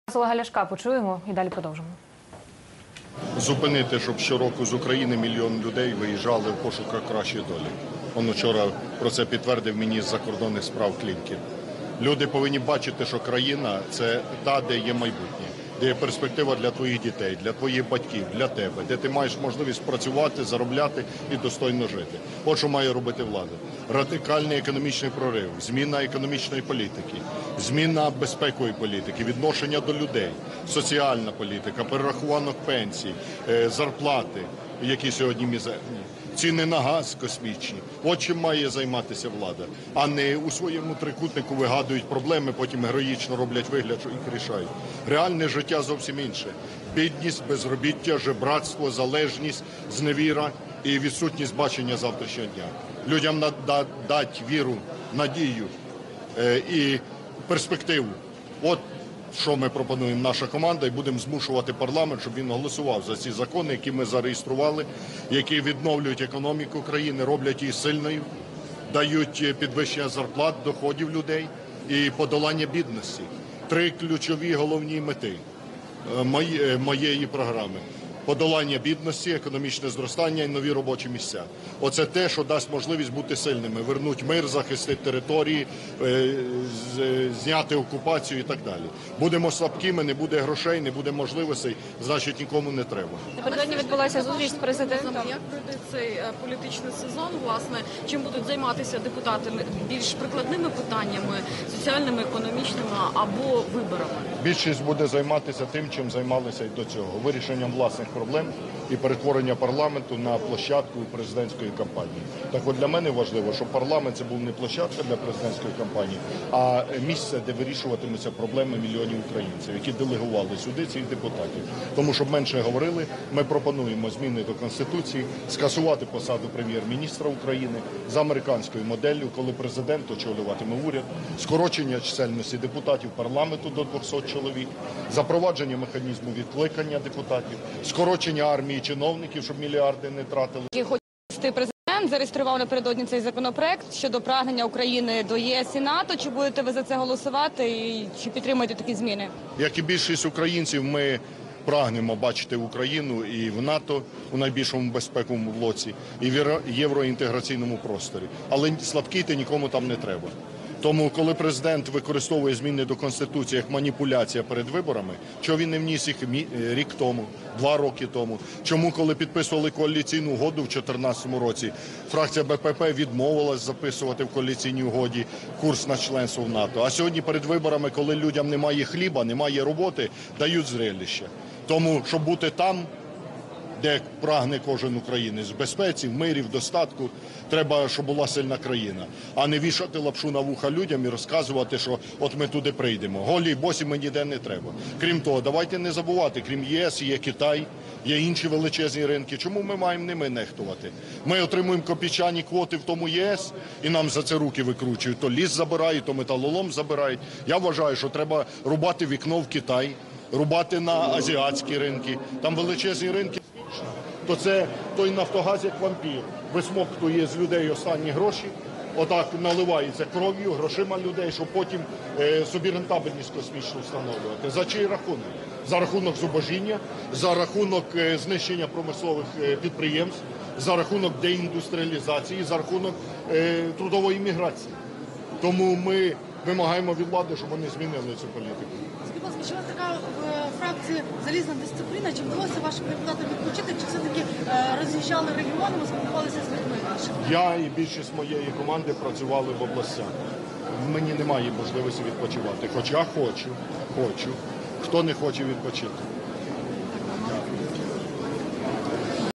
Ляшко: У людей немає хліба, тому влада дає їм «зрєліща»
Парламент повинен бути місцем, де вирішуються проблеми мільйонів українців, а не майданчиком для передвиборчої агітації. На цьому наголосив лідер Радикальної партії Олег Ляшко у кулуарах парламенту. Політик стверджує, найбільші проблеми людей сьогодні – це бідність, безробіття і зневіра, через що мільйони українців виїжджають за кордон на заробітки.
«Люди повинні бачити, що в країні є майбутнє, є перспективи для дітей, є можливість працювати, заробляти і достойно жити. Радикальний економічний прорив, зміна безпекової та соціальної політики, перерахунок пенсій і зарплат, які сьогодні мізерні, зниження «космічних» цін на газ – ось чим має займатися влада, а не у своєму трикутнику вигадувати проблеми і потім героїчно робити вигляд, що їх вирішує. Реальне життя зовсім інше: бідність, безробіття та зневіра. Людям треба дати віру, надію і перспективу!», - зауважив Ляшко.
Лідер РПЛ пропонує план порятунку економіки, який дасть можливість подолати бідність і повернути українців додому.
«Три ключові мети моєї програми: подолання бідності, економічне зростання і нові робочі місця. Це те, що дасть можливість бути сильними, повернути мир, захистити території, зняти окупацію. Якщо ми будемо слабкими – ми нікому не будемо потрібні. Але більшість депутатів цього не розуміє. Вони і надалі вирішуватимуть власні проблеми і перетворюватимуть парламент на майданчик президентської кампанії. Для мене важливо, щоб парламент був місцем, де вирішуватимуться проблеми мільйонів українців. Тому ми пропонуємо скоротити чисельність депутатів до 200 чоловік, запровадити механізм відкликання депутатів, скоротити армію чиновників, щоб мільярди не витрачали на них, а також скасувати посаду прем'єр-міністра України за американською моделлю, щоб президент очолював уряд», - підкреслив Ляшко.